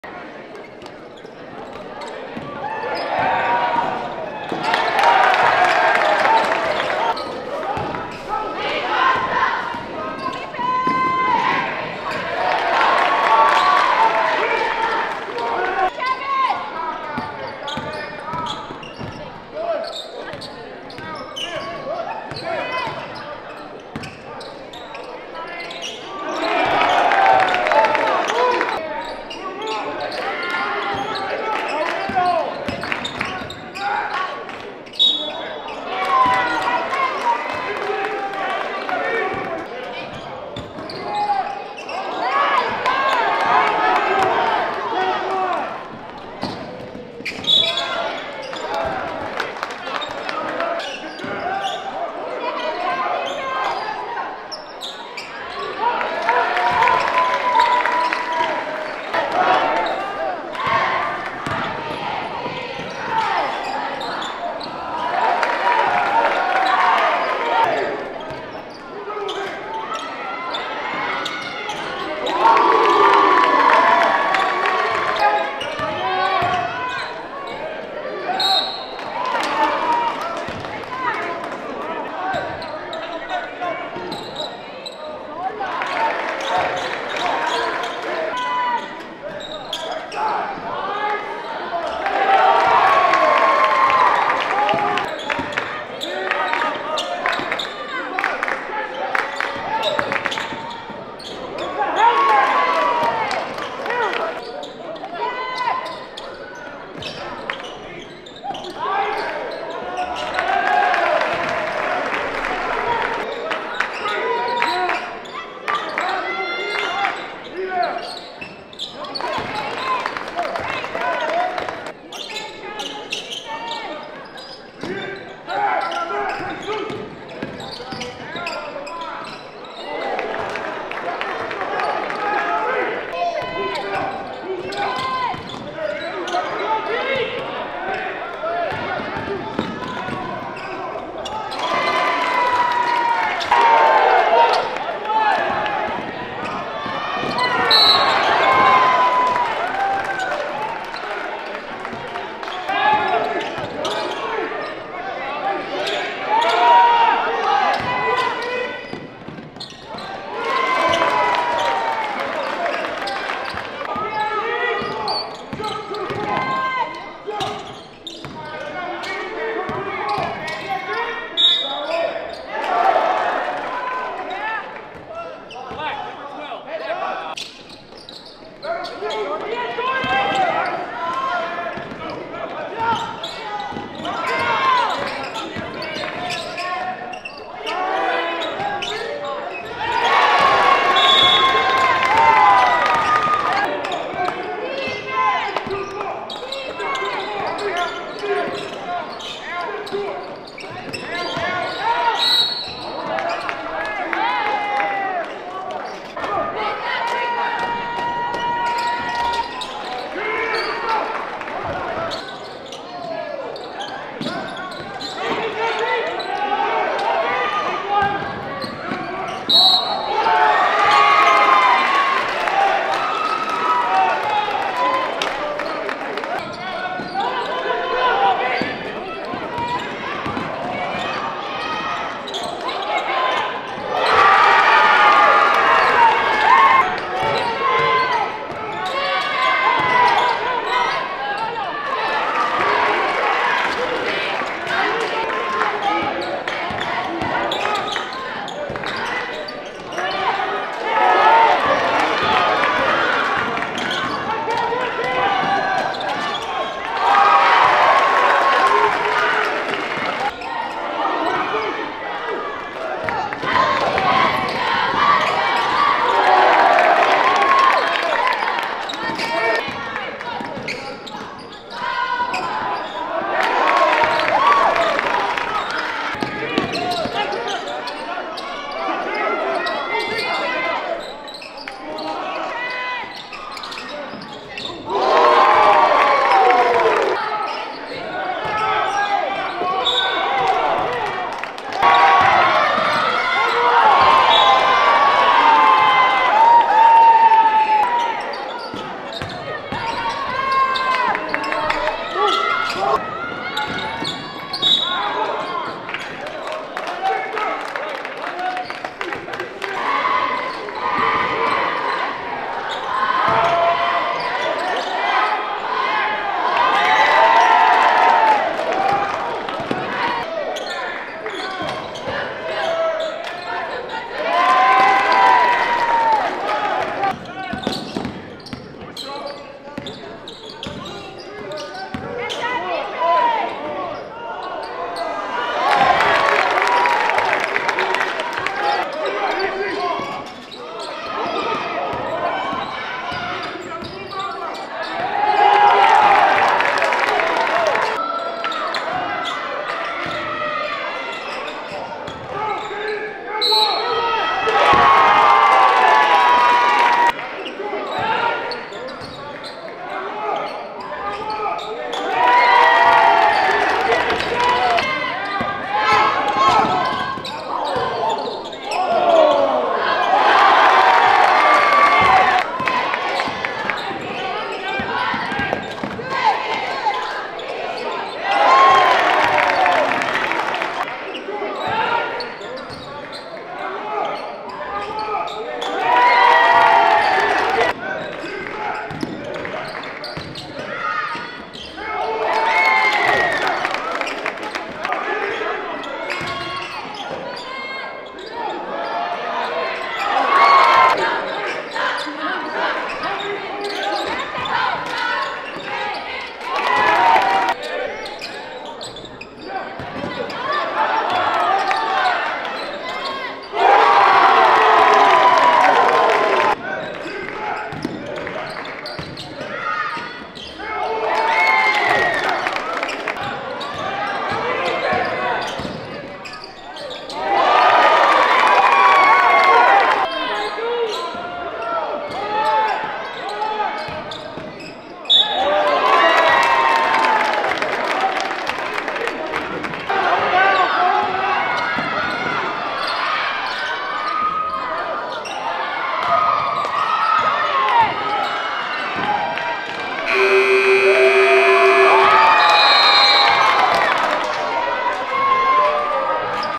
Chavez vs. Bear Creek - Full Game Highlights
1st League Game - January 4, 2023
Bear Creek Wins 58-56

Your Source For Central Valley High School Sports